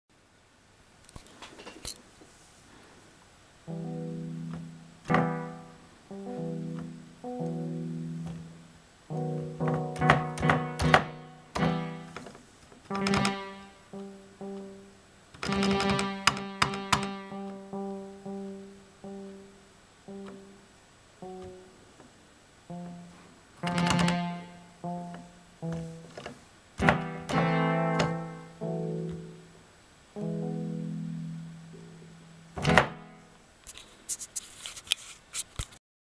stm32 + diode matrix = midi keyboard with velocity sensing
This was just a quick and dirty "proof of concept". The main project was to read out a 76 key vintage yamaha YP-40 clavinova claviature.